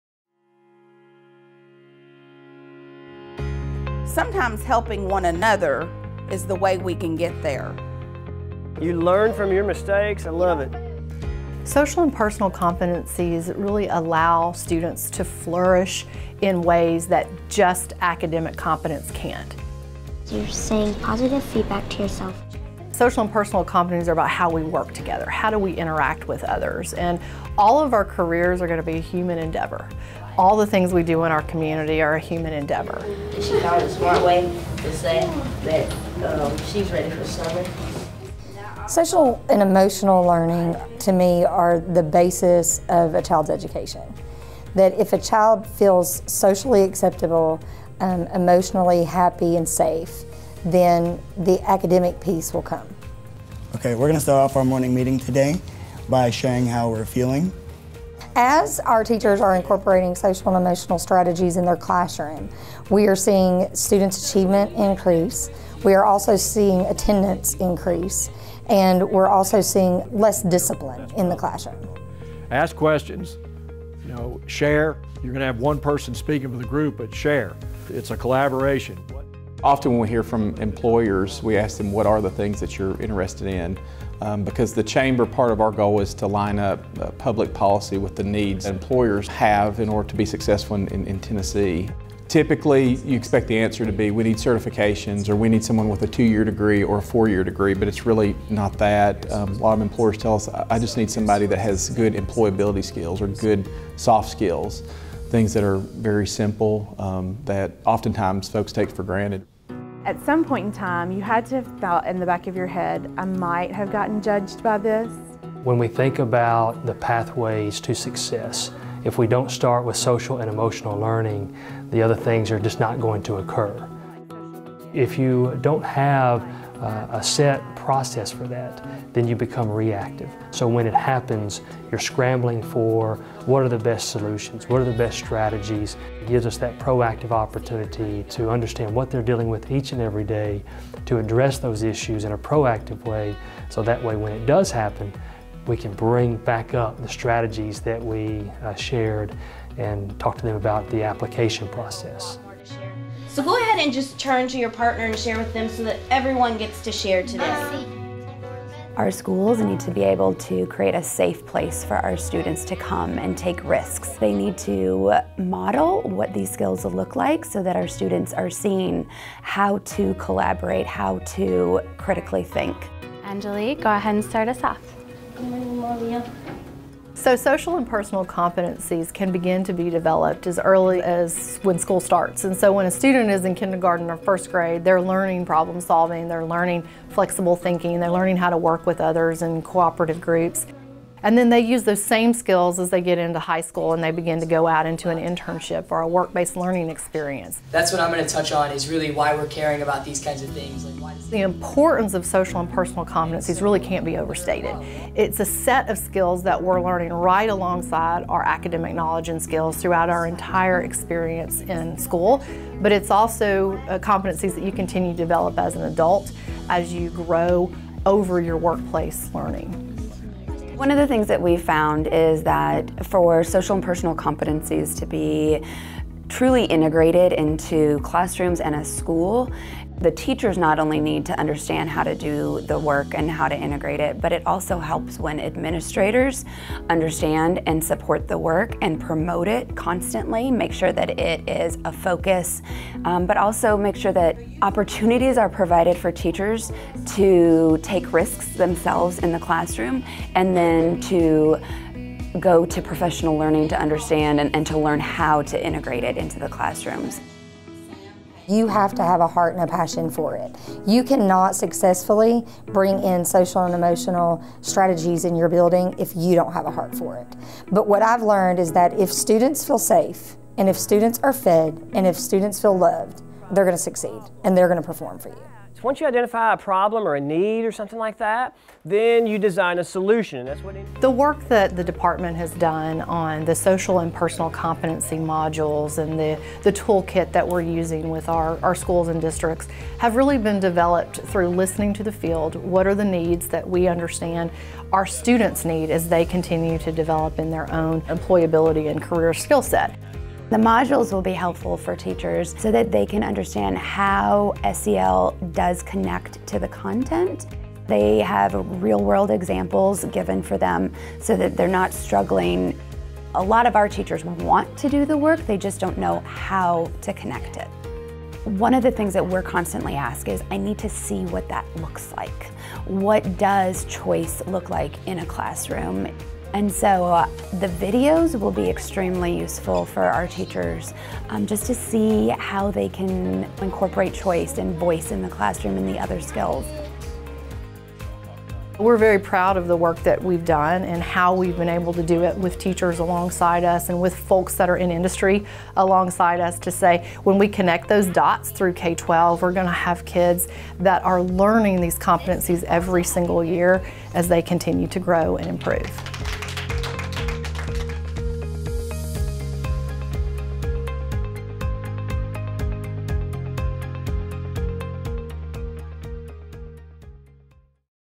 Overview of Social and Personal Competencies
State and district educators as well as business leaders share the importance of social and personal competencies for success both in school and beyond. This video was developed for the Tennessee Department of Education.